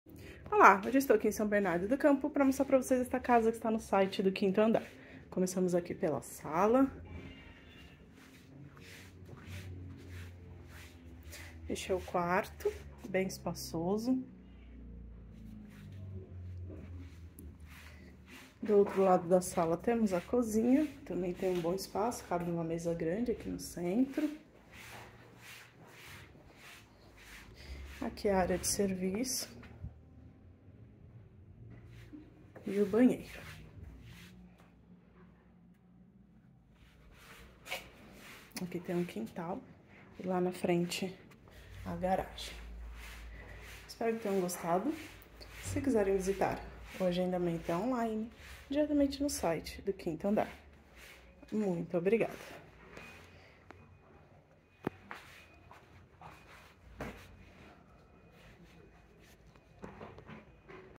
QuintoAndar imóvel 1629642
Casa com 1 dormitório, 200m².
Localizado na Rua Tibiriçá, SBC